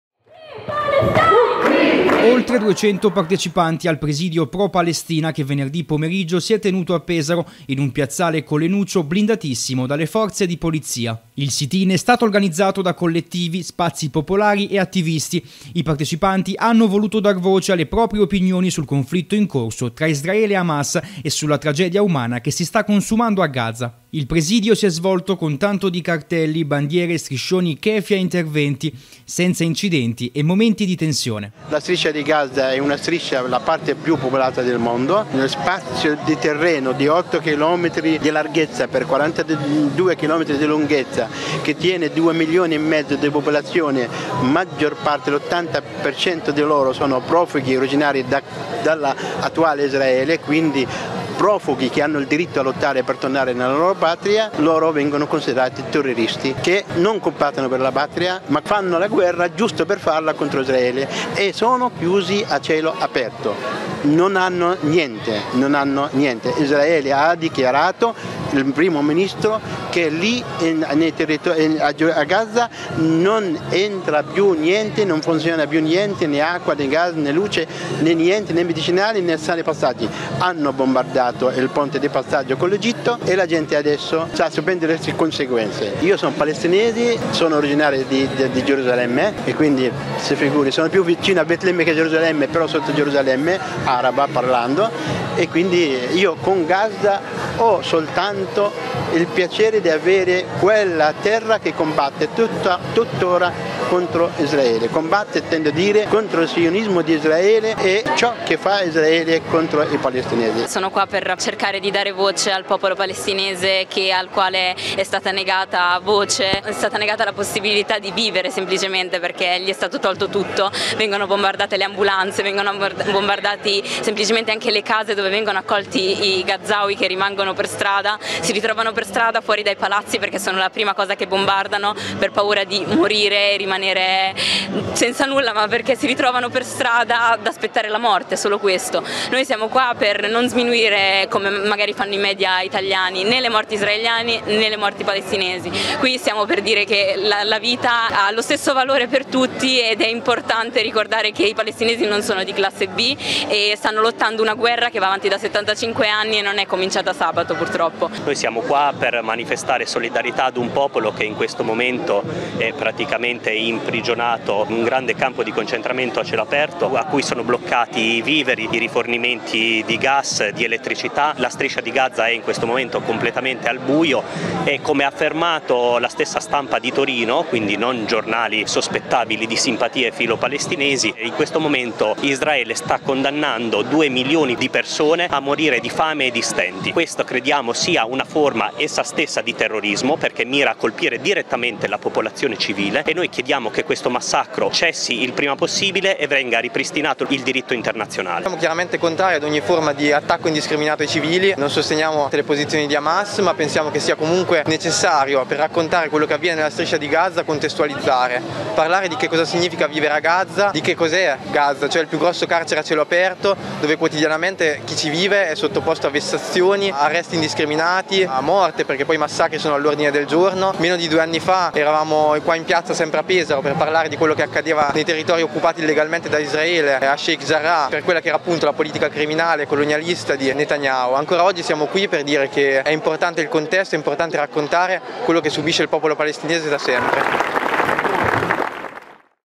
Manifestazione di solidarietà al popolo palestinese
In Piazzale Collenuccio a Pesaro, dove venerdì scorso si è svolta una manifestazione di solidarietà nei confronti del popolo palestinese.